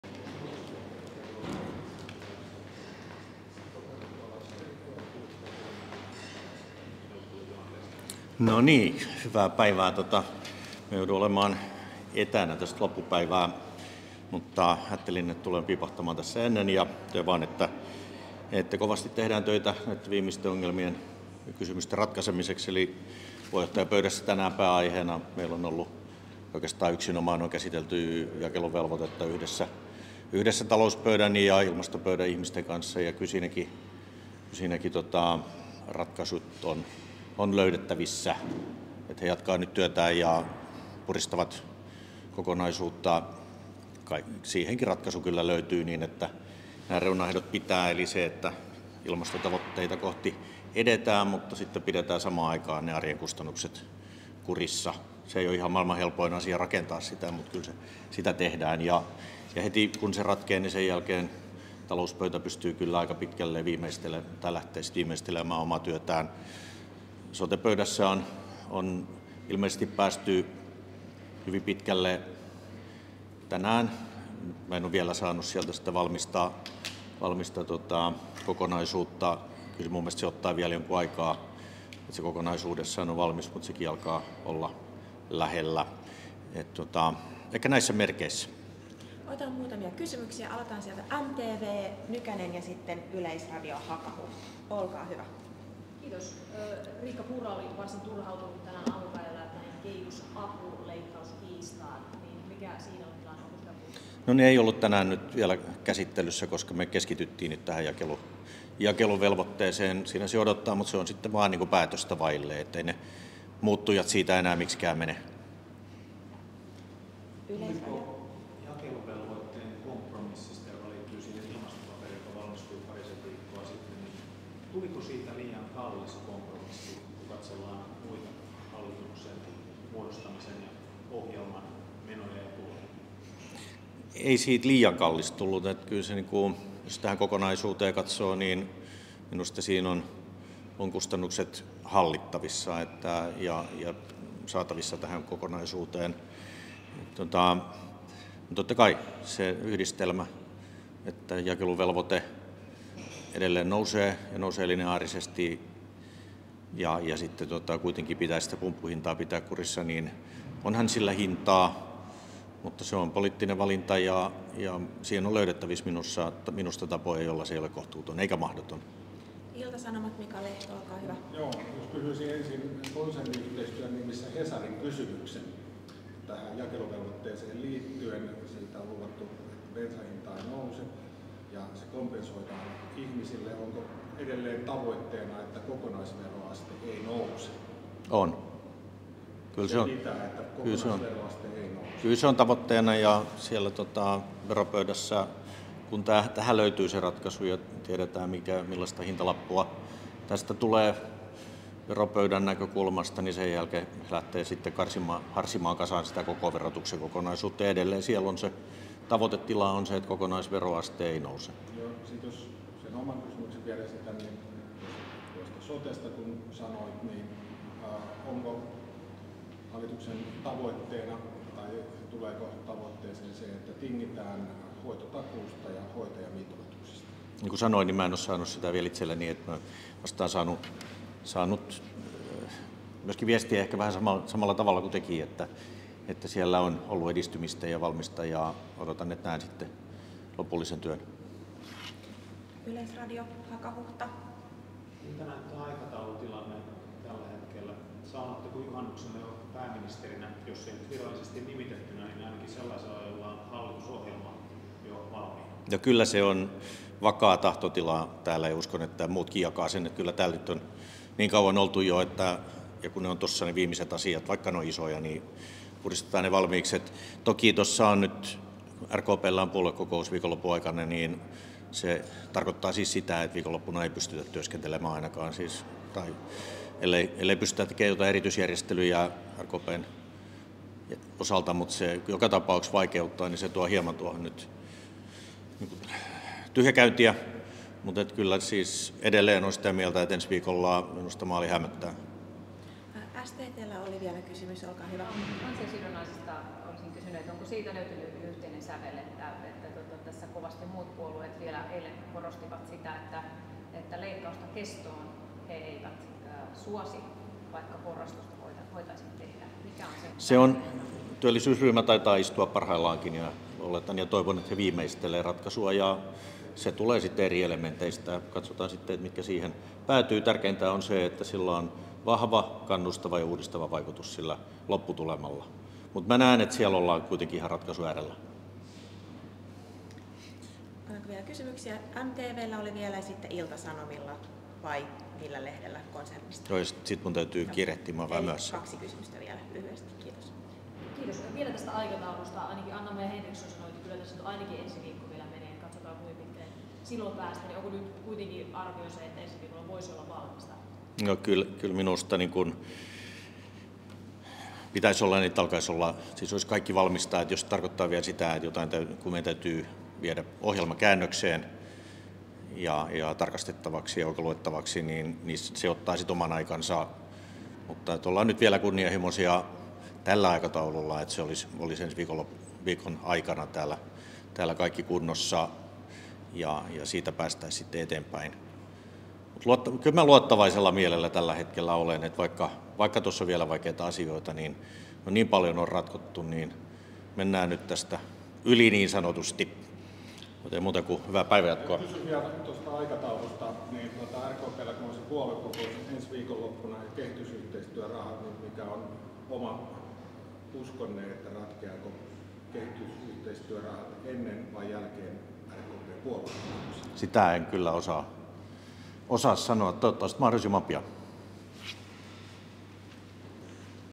Hallituksen muodostaja Petteri Orpon kommentit 7.6.
Hallituksen muodostaja Petteri Orpo vastasi median kysymyksiin  Säätytalossa keskiviikkona 7.6.